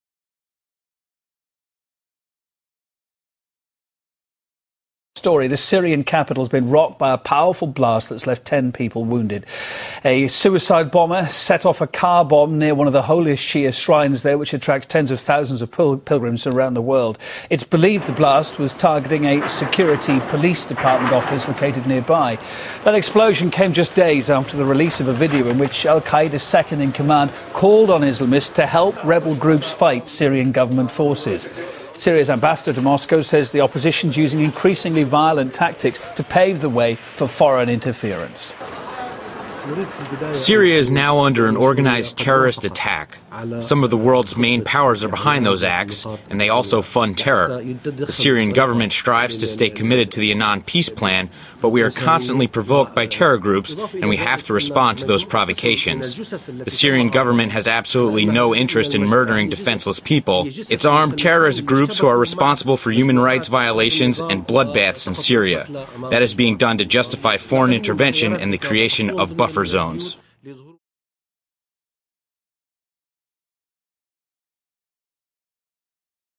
Syrian ambassador to Russia: "Syria is now under attack from the Worlds largest powers"!!
Syrian ambassador to Russia, Riyad Haddad says that terror attacks are now being funded by western countries and are being performed in Syria.